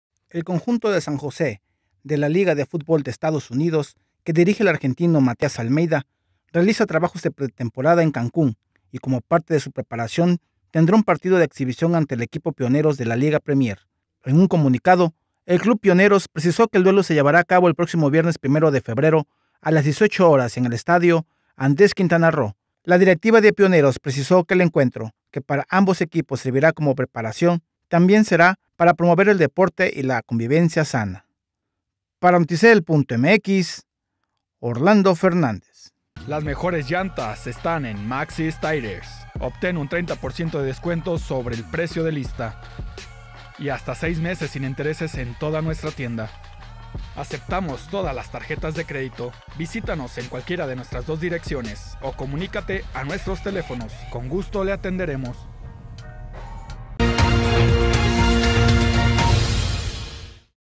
Almeyda hace pretemporada
Almeyda y su equipo San Jose de MLS realiza pretemporada en Cancún